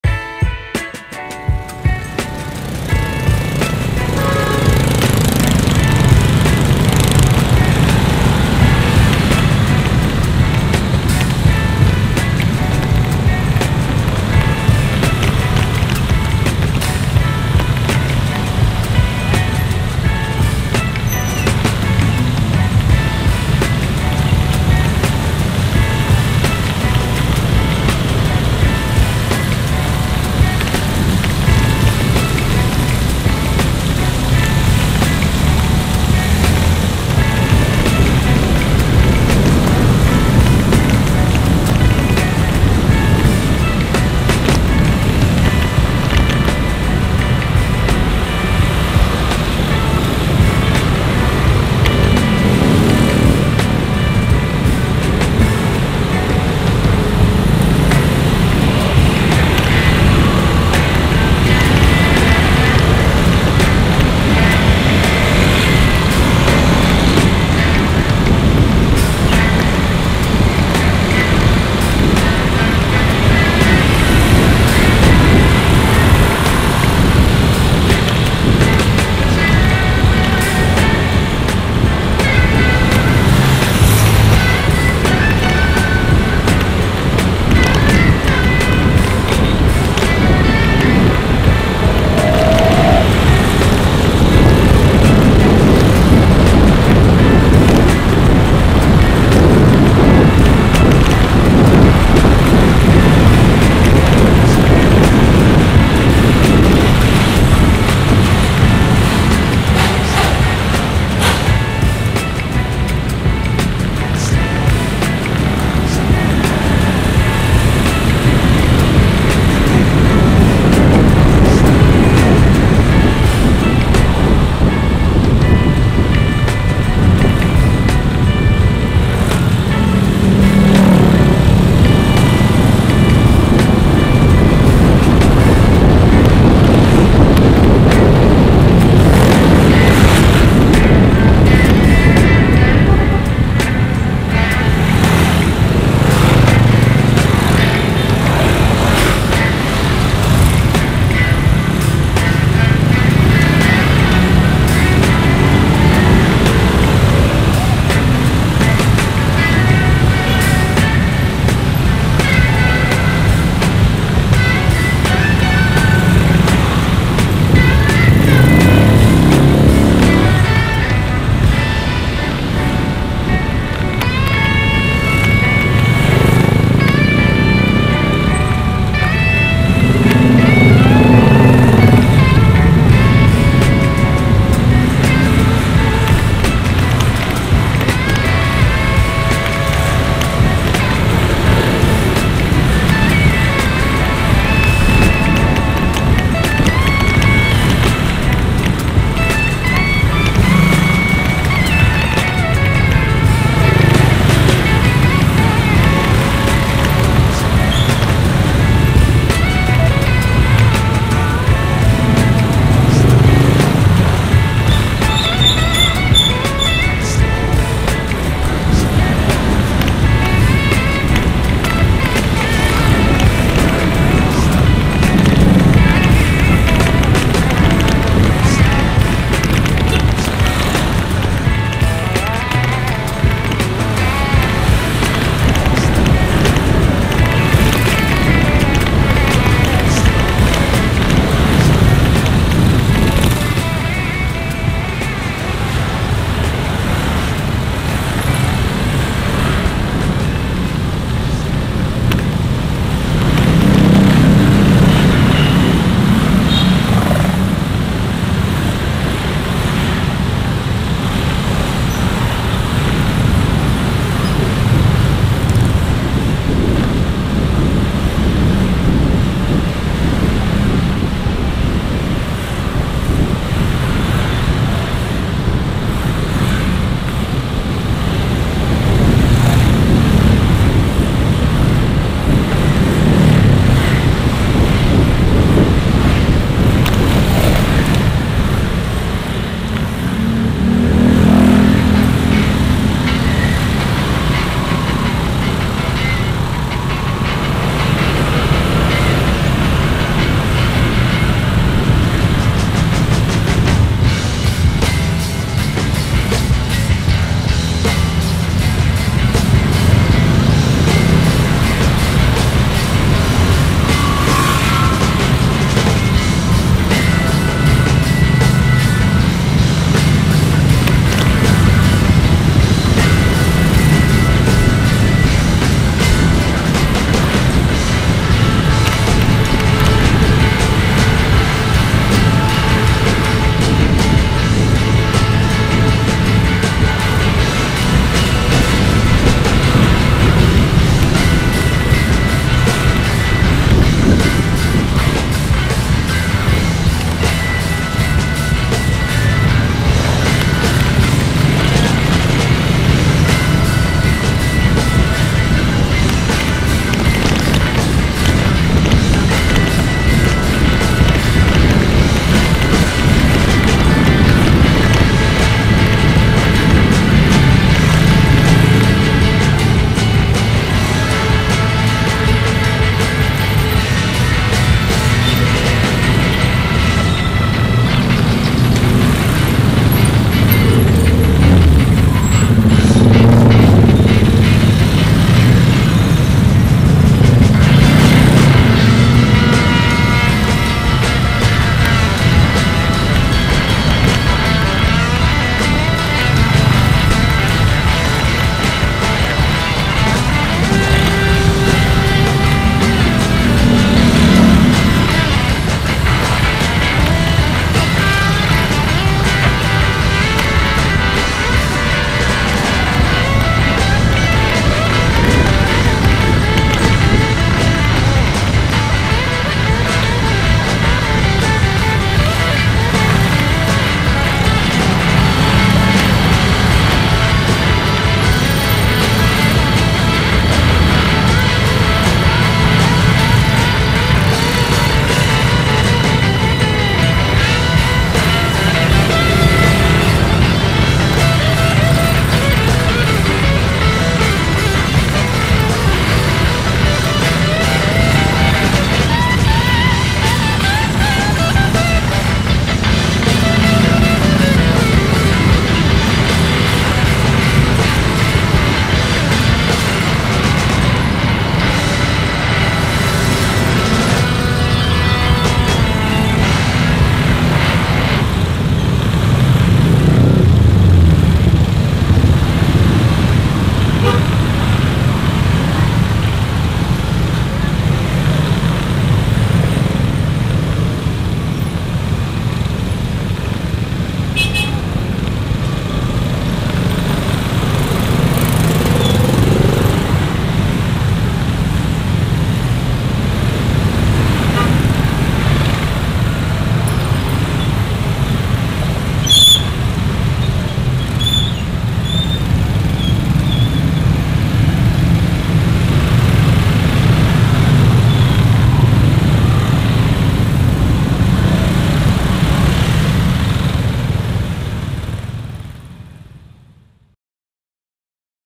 Perempatan SGC Menuju Kampung Pilar//Jalan Sempit// Moto Vlog
kali ini sy berbagi Vidio moto vlog
start perempatan SGC menuju Jln Pilar(gang Sarbat) lanjut ke Jln jagawana

terimakasih 🙏